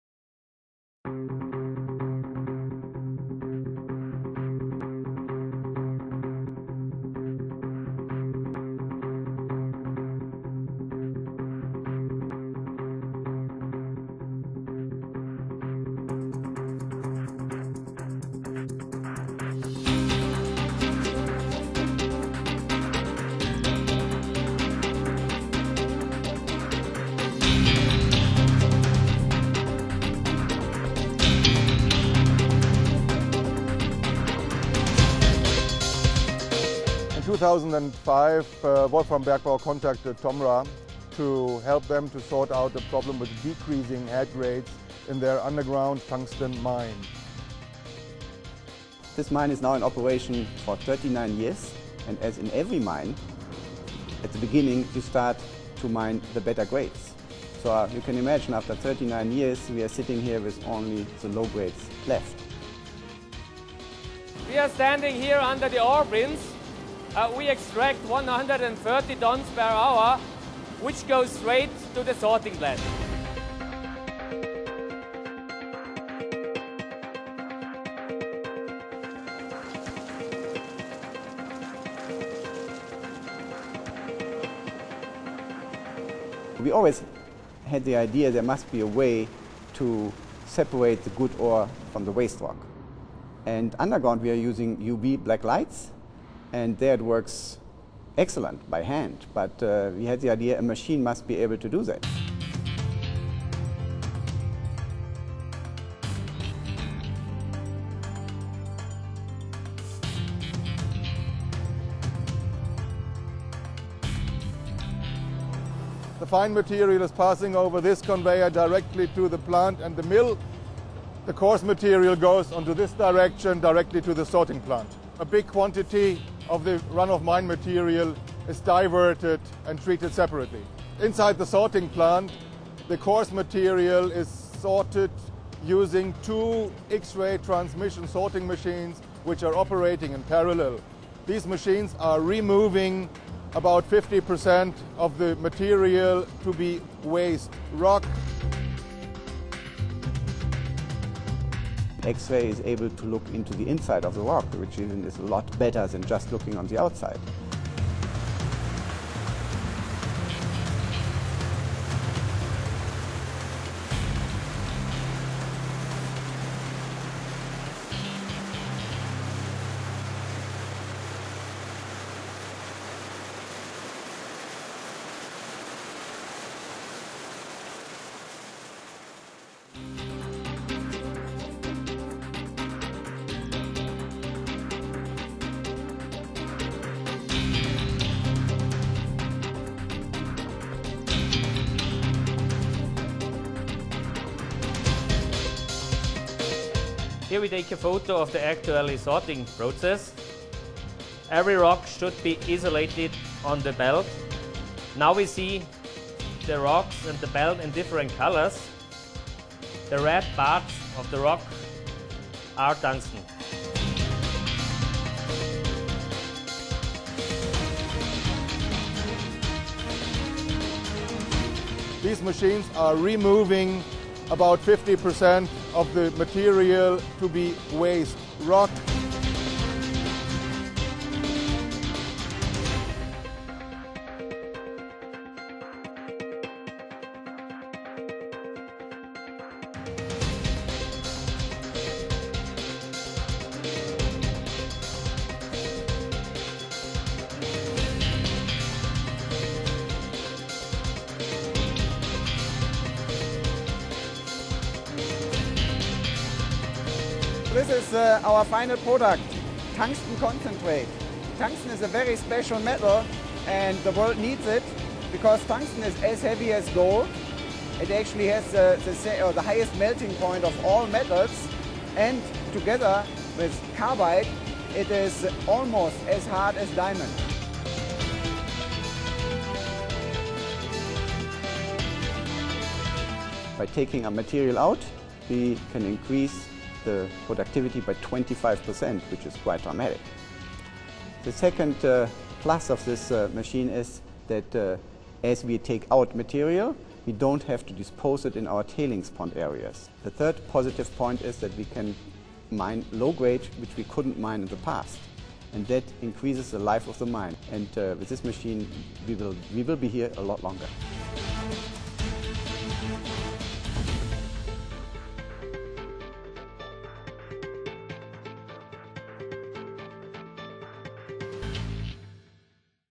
Tungsten Sorting at WOLFRAM Bergbau AG, Austria
Tungsten plays a large and indispensable role in modern high-tech industry. Up to 500,000 tons of raw tungsten ore are mined each year by Wolfram Bergbau und Hütten AG (WHB)in Felbertal, Austria, which is the largest scheelite deposit in Europe.

More than 100,000 tons per year do not need to be processed and disposed as ﬁne tailings into settling ponds with limited capacity. The XRT-sorted waste rock can instead be sold as aggregates for road construction.